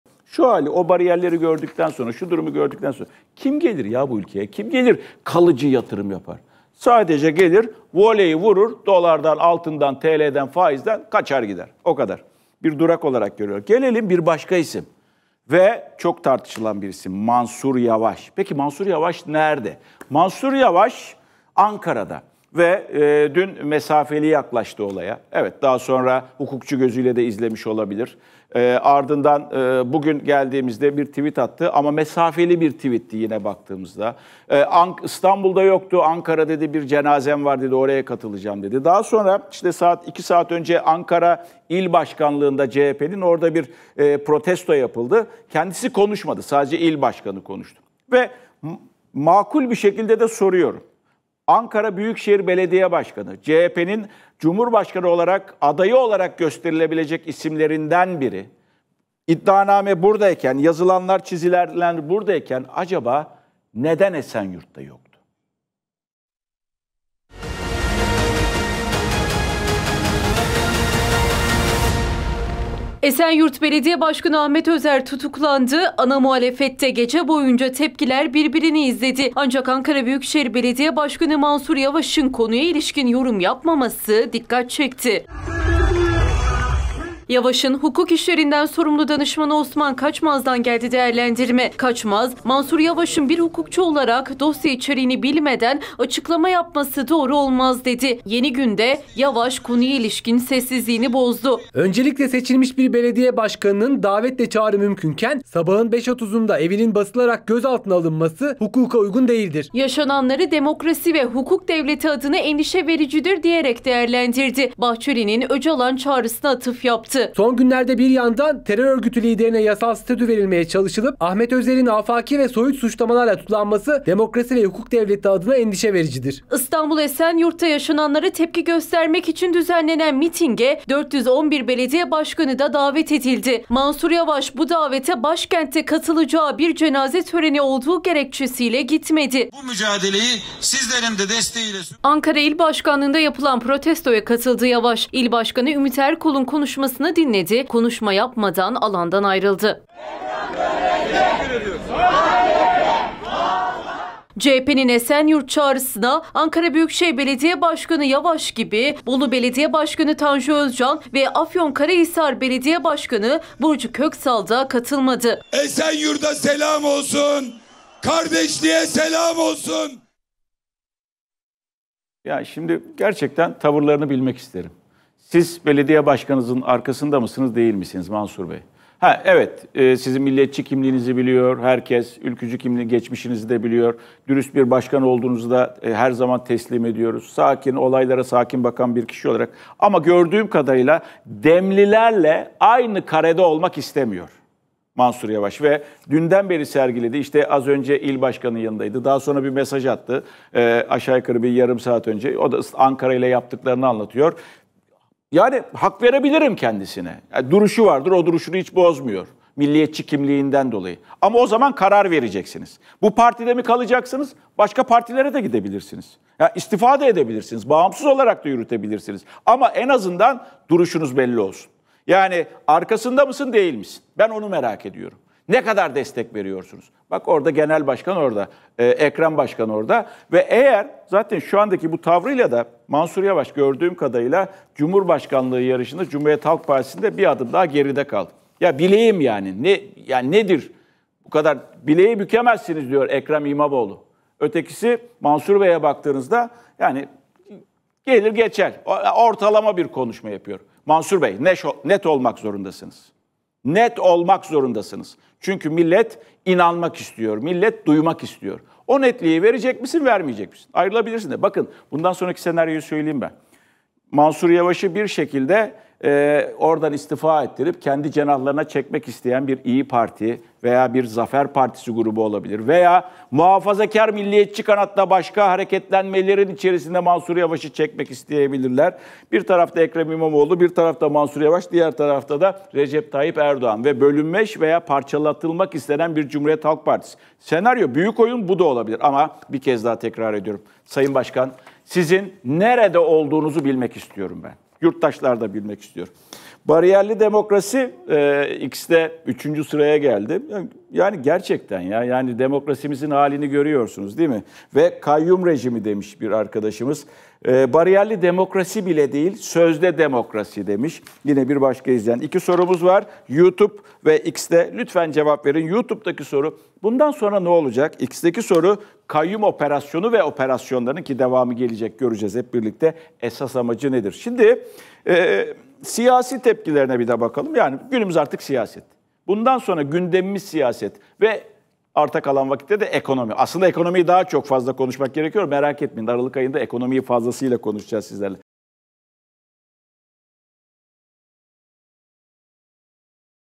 Mansur Yavaş Esenyurt'a Gitmedi Fatih Portakal'dan Olay Yorum Geldi! 'Yavaş'ı İstifa Ettirip...'
Digiturk: 60
D-smart: 93
Turkcell TV Plus: 69
Tivibu: 69
Türksat Kablo TV: 76